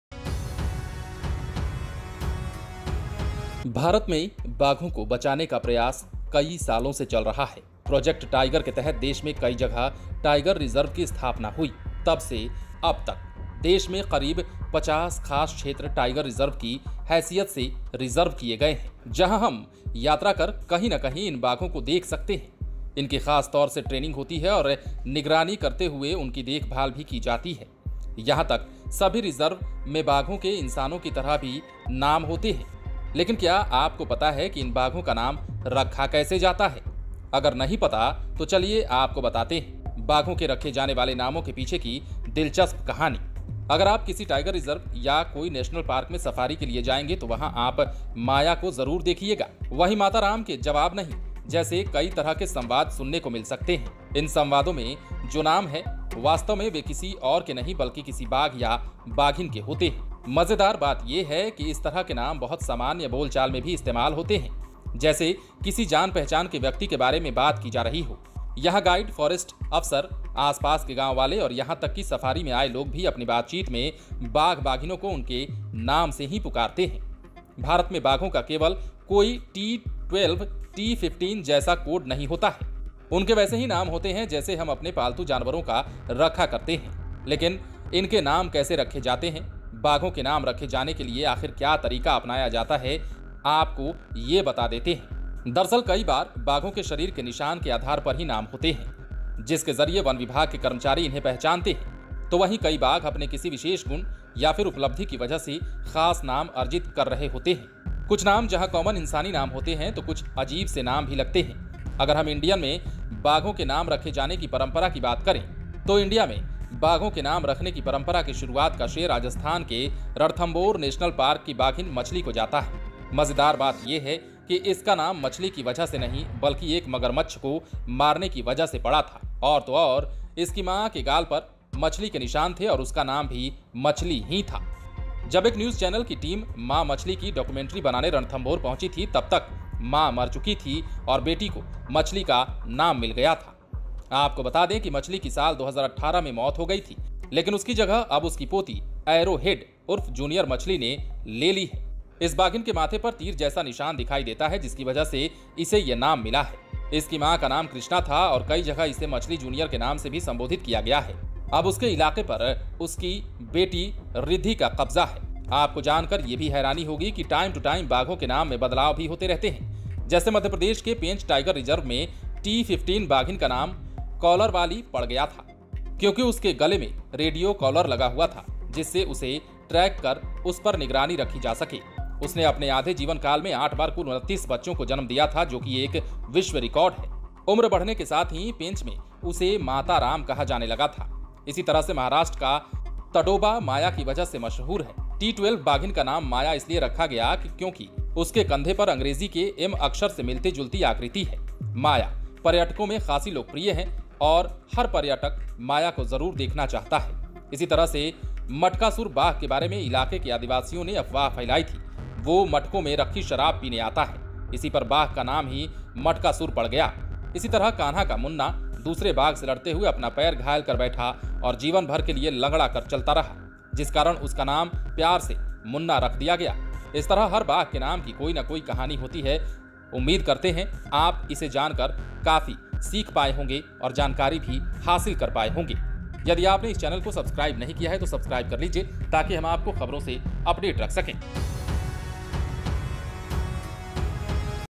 India में ऐसे रखे जाते हैं Tigers के नाम, जानिए क्या है इसके पीछे की कहानी, देखिए
क्या आप जानते हैं भारत में कैसे रखे जाते हैं बाघों के नाम? बड़ी दिलचस्प है कहानी, देखिए वीडियो...